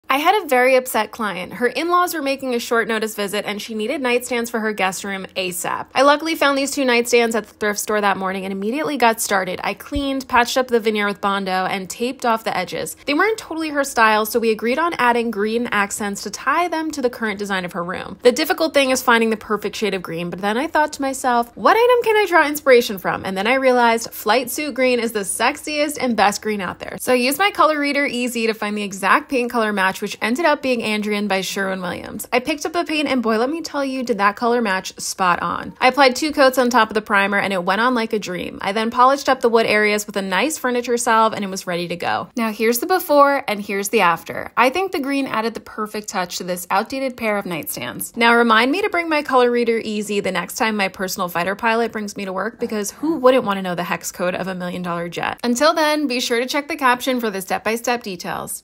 I had a VERY Upset Client.... Quick Painting of Thrifted Nightstands!
Here is the list of items used to flip these nightstands

And here is the link to the ColorReader EZ!